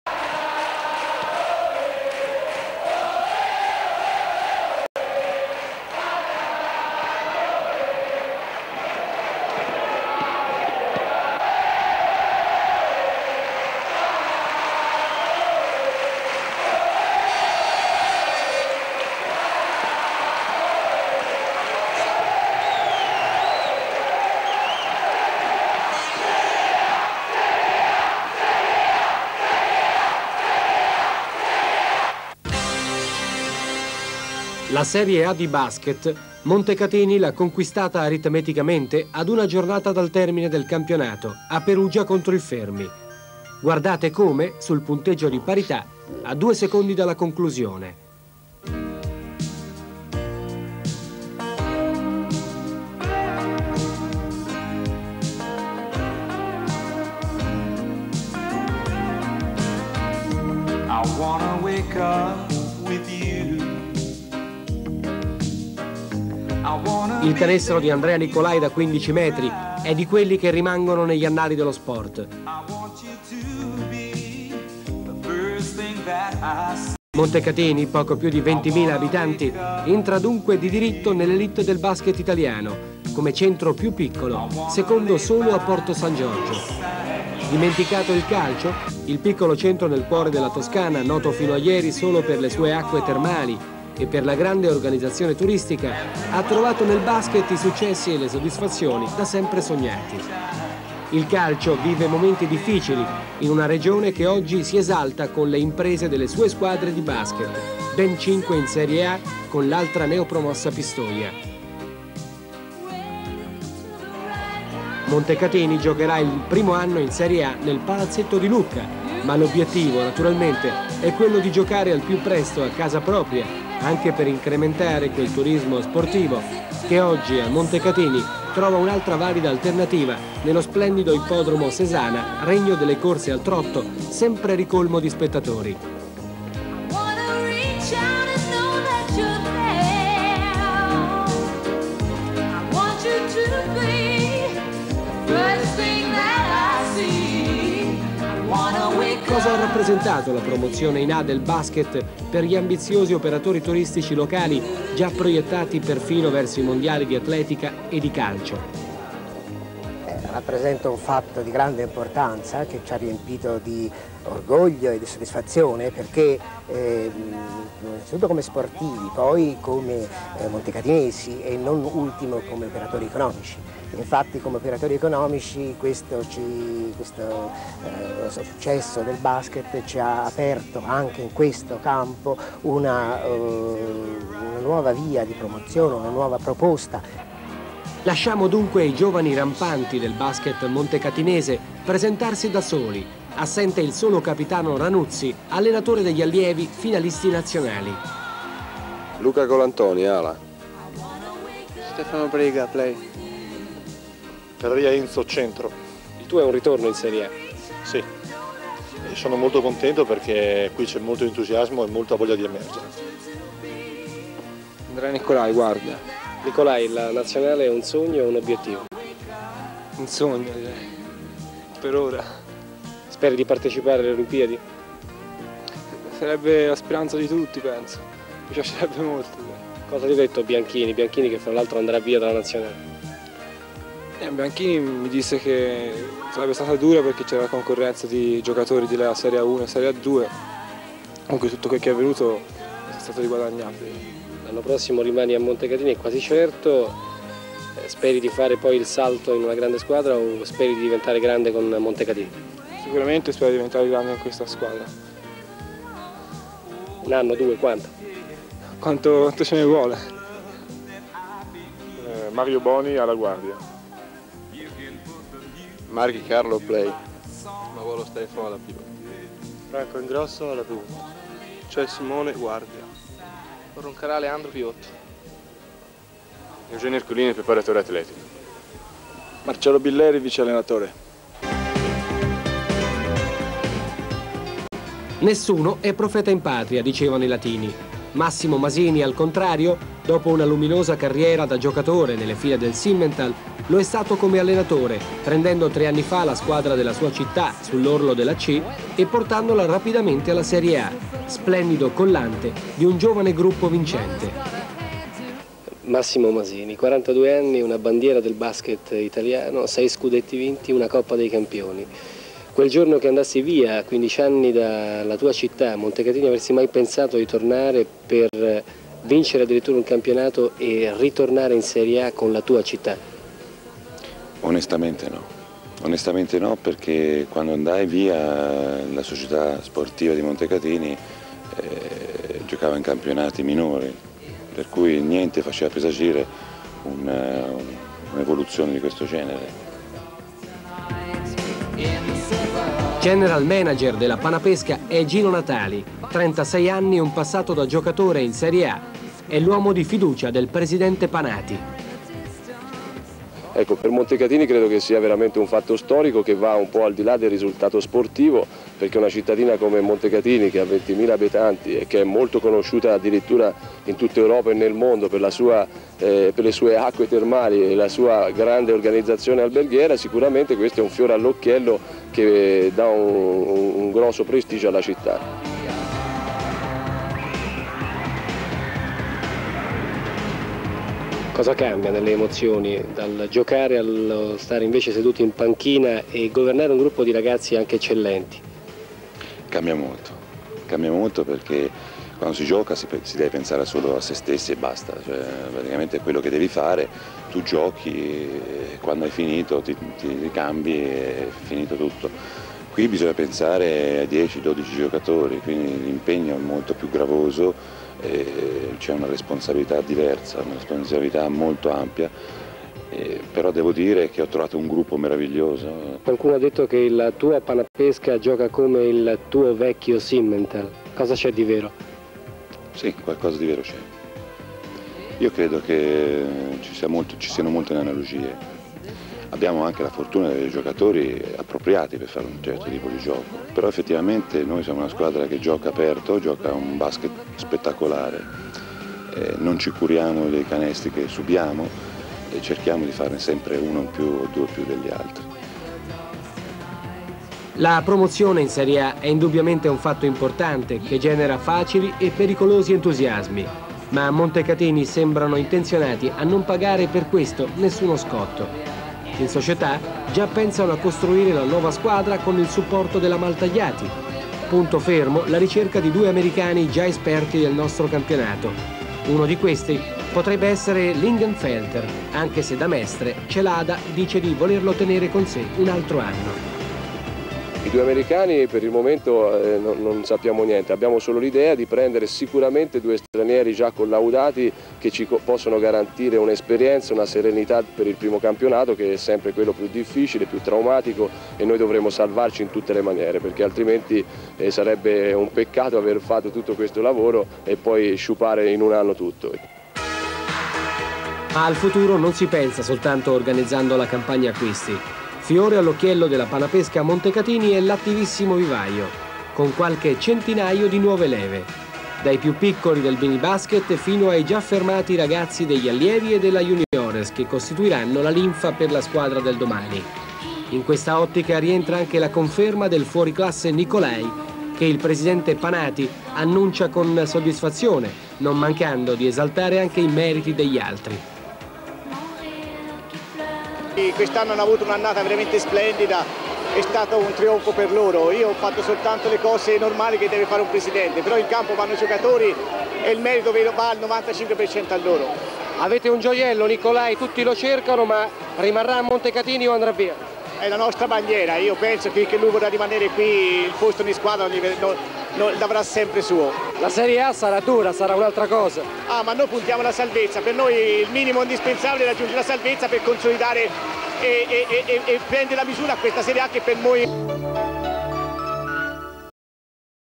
Promozione del Montecatini Basket in serie A2 (1987).
Servizio di Franco Lauro